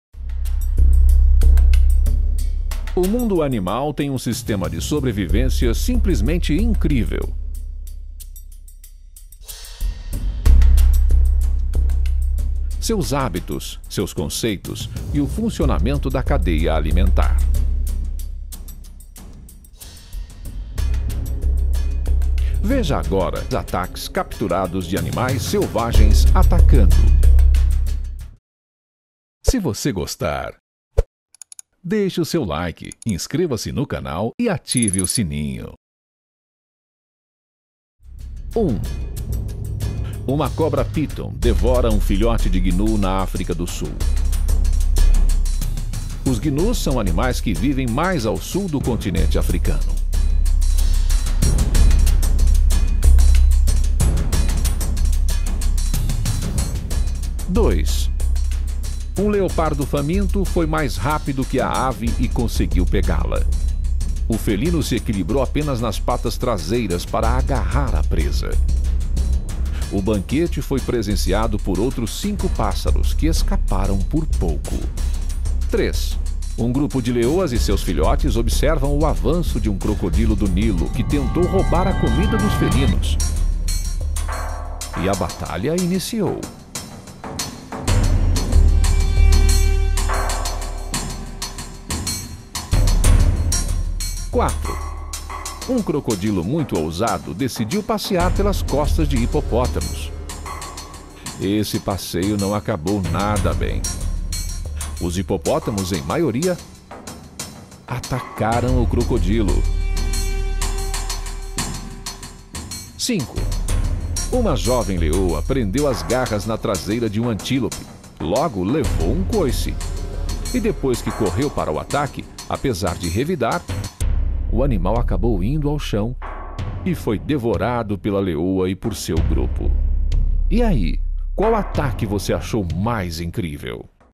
5 ATAQUES DE ANIMAIS MAIS INCRÍVEIS DO MUNDO
O mundo animal tem um sistema de sobrevivência simplesmente incrível. Seus hábitos, seu conceitos, e o funcionamento da cadeia alimentar.

Veja agora 6 ataques capturados de animais selvagens atacado:

Uma cobra píton devora um filhote de gnu na África do Sul. Os gnus são animais que vivem mais ao sul do continente africano.

Um leopardo faminto foi mais rápido que a ave e conseguiu pegá-la. O felino se equilibrou apenas nas patas traseiras para agarrar a presa. O banquete foi presenciado por outros cinco pássaros, que escaparam por pouco.

Um grupo de leoas e seus filhotes observam o avanço de um crocodilo-do-nilo que tentou roubar a comida dos felinos, e a batalha início.

Um crocodilo muito ousado decidiu passear pelas costas de hipopótamos. Esse passeio não acabou nada bem, os hipopótamos em maioria atacaram o crocodilo.

Uma jovem leoa prendeu as garras na traseira de um antílope, logo levou um coice. E depois que correu para o ataque, apesar de revidar, o animal acabou indo ao chão e foi devorado pela leoa e por seu grupo.

E aí, qual ataque você achou mais incrível?